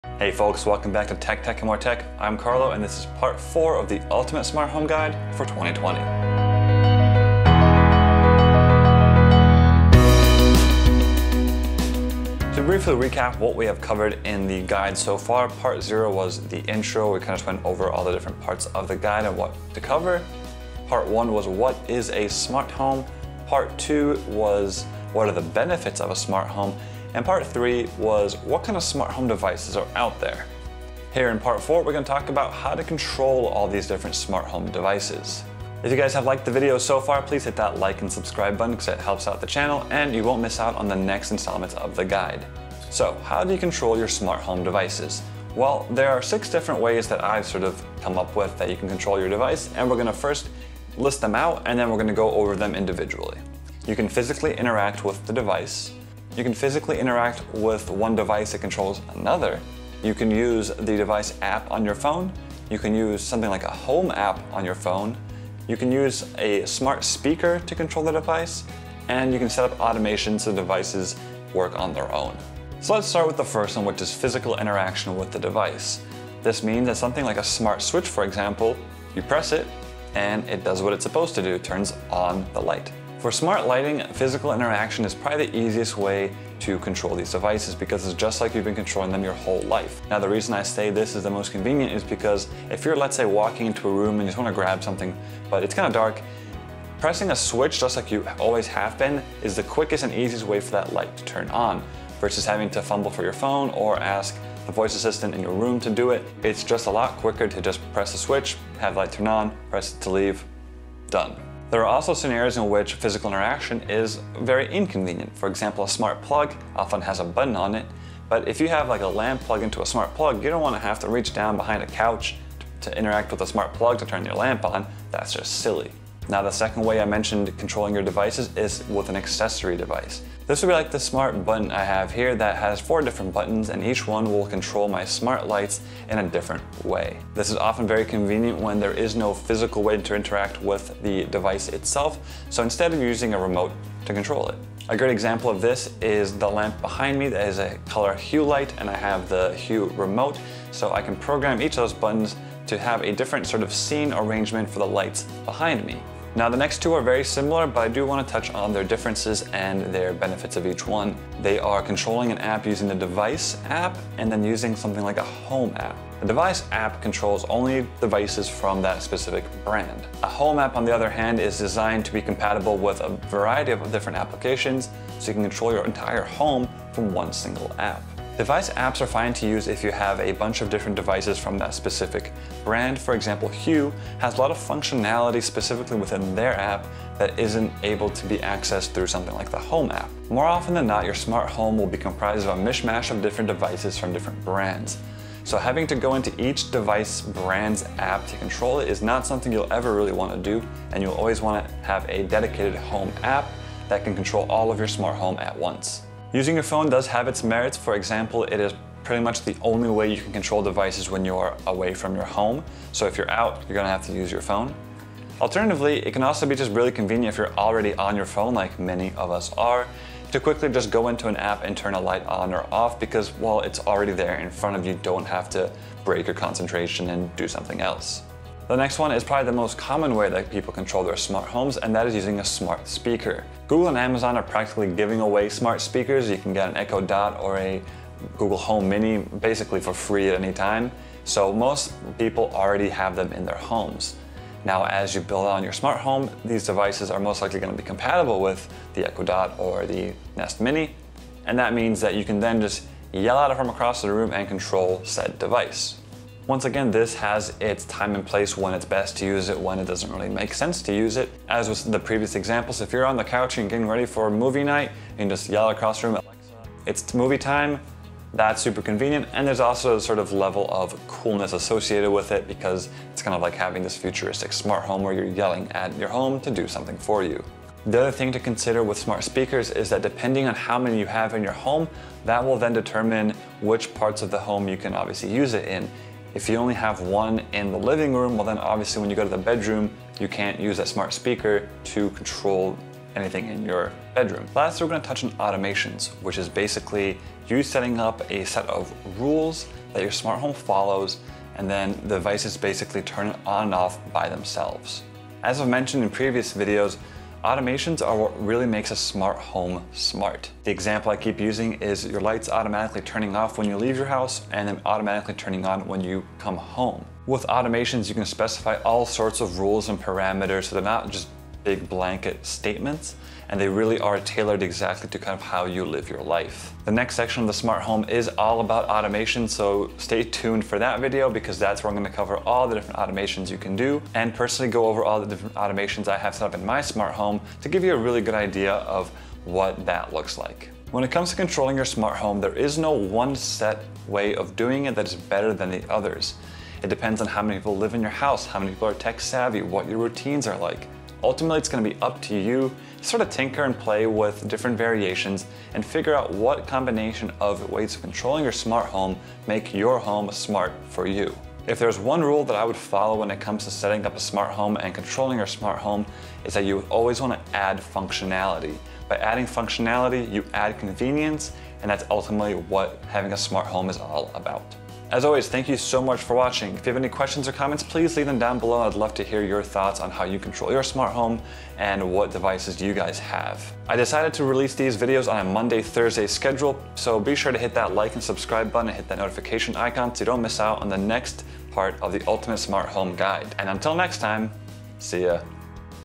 How to Control Your Smart Home! - ULTIMATE Smart Home Guide 2020 Part 4
Now that you know what kind of Smart Home devices you can introduce into your home, you'll need a way of effectively controlling them.
This part of the Ultimate Smart Home Guide will discuss the various ways of controlling your Smart Home.